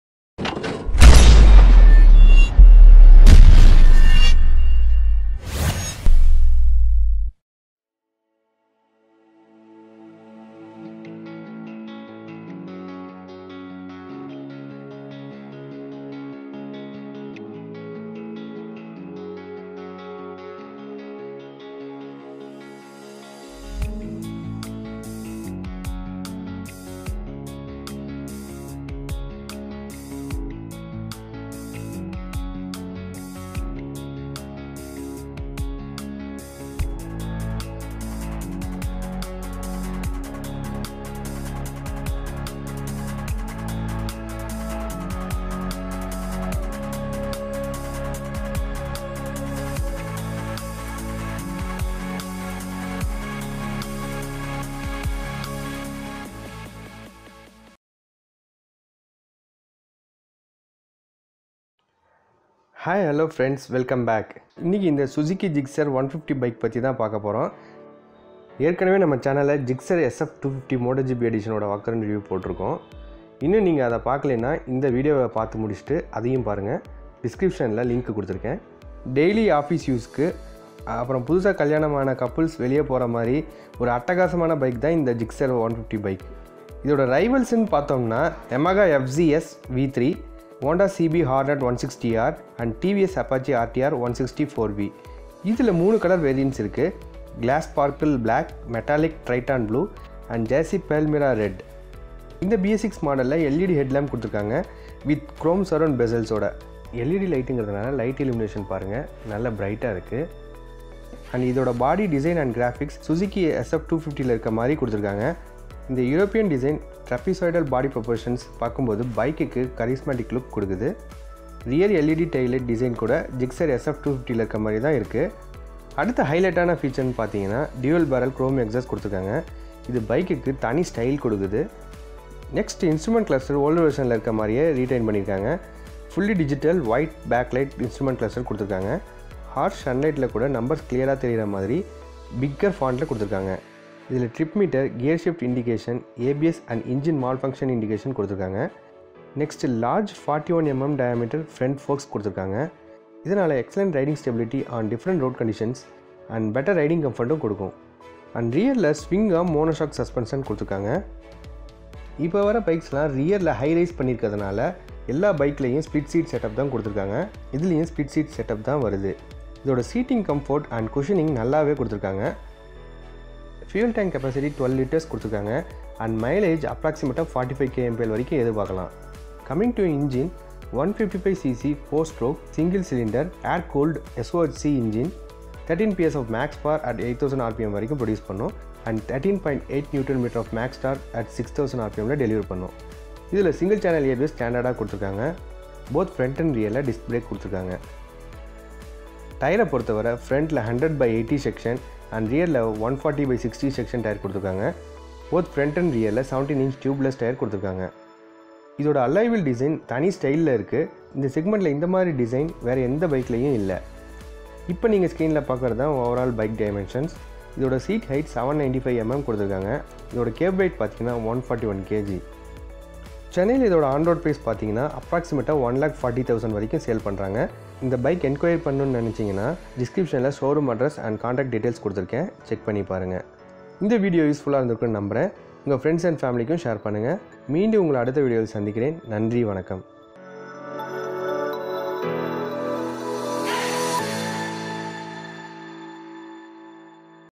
Gixxer 155 BS6 | Gixxer 155 bs6 review in Tamil
Gixxer 155 BS6 | In this video,let see the features ,color variant and on road price of Gixxer 155 bs6.

Mr. Sekar
#99, SHIV MISHRI MANSION MOUNT PONAMALLEE, Trunk Rd, Porur, Chennai, Tamil Nadu 600116